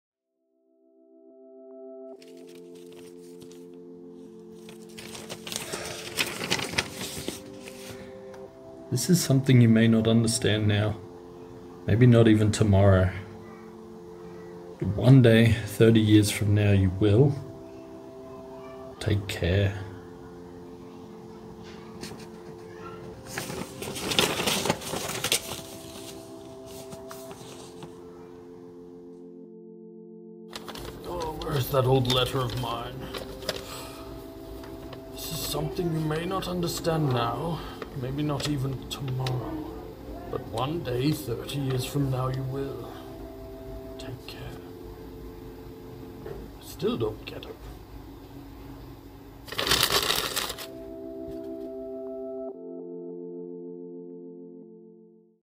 30 Years Later - One Minute Short Film
Have you ever been told, " You'll understand when you're older."?

This Short film was made entirely with the Samsung Galaxy S23 Ultra.